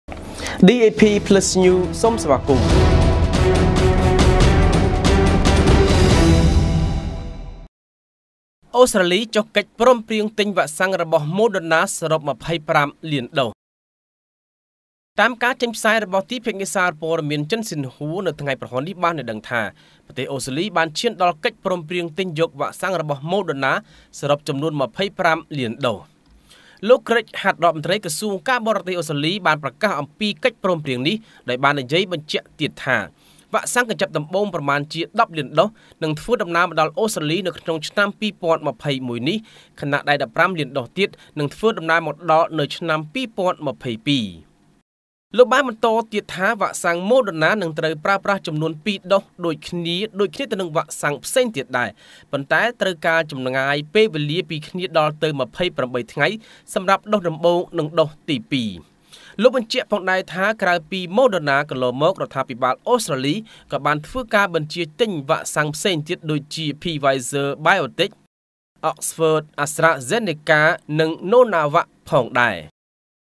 អូស្ត្រាលី ចុះកិច្ចព្រមព្រៀង ទិញវ៉ាក់សាំងរបស់ Moderna សរុប២៥លានដូស
យោងតាមការចេញផ្សាយ របស់ទីភ្នាក់ងារសារព័ត៌មាន ចិនស៊ិនហួ នៅថ្ងៃព្រហស្បតិ៍នេះ បានឲ្យដឹងថា ប្រទេសអូស្ត្រាលី បានឈានទៅដល់កិច្ចព្រមព្រៀង ទិញយកវ៉ាក់សាំងរបស់ Moderna សរុបចំនួន ២៥លានដូស។